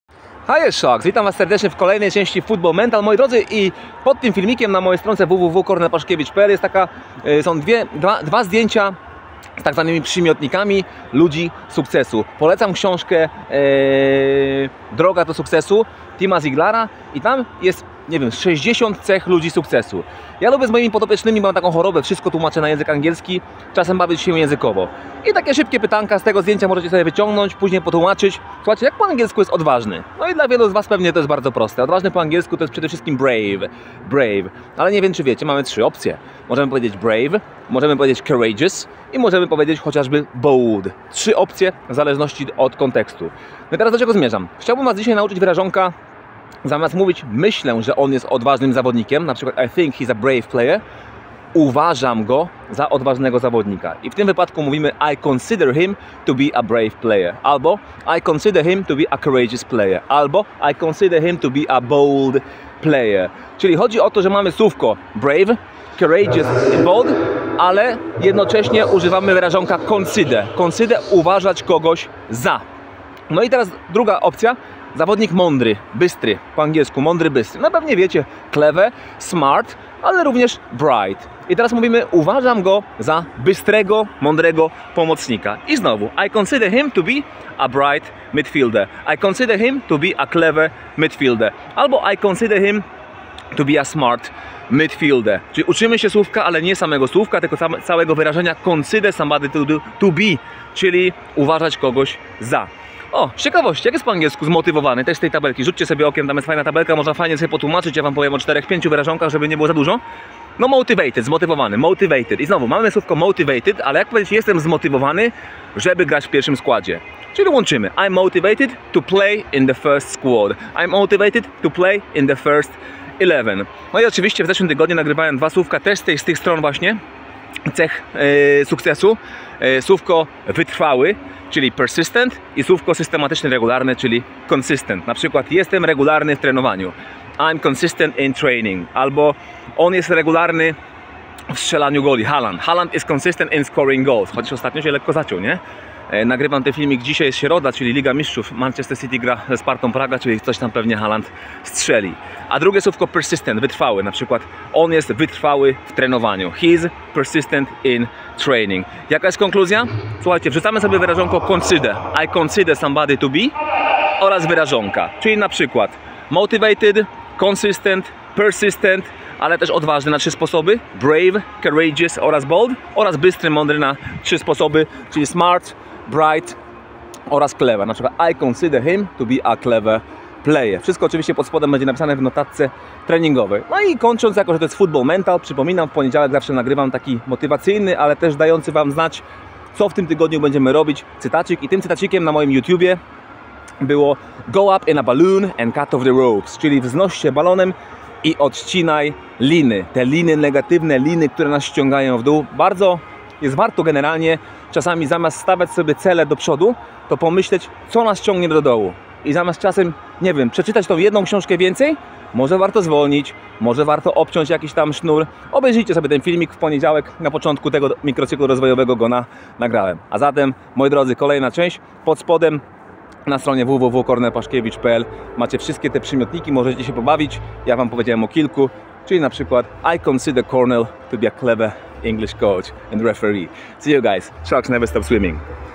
FOOTBALL MENTAL 15 (s.24/25) - Wybierz SUKCES (consider).
Zabawa językowa z CECHAMI SUKCESU w wyrażeniu "I consider him to be a COURAGEOUS / BRIGHT / MOTIVATED / CONSISTENT player". Czytaj książki, ucz się angielskiego, rozwijaj się niczym BESTIA. Zapraszam.

Step by Step. Kornel THE SHARK Paszkiewicz 😊

‼️⚽🦈 BUDUJEMY POLSKĄ PIŁKĘ ‼️⚽🦈

Step by Step. Kornel 🦈🦈🦈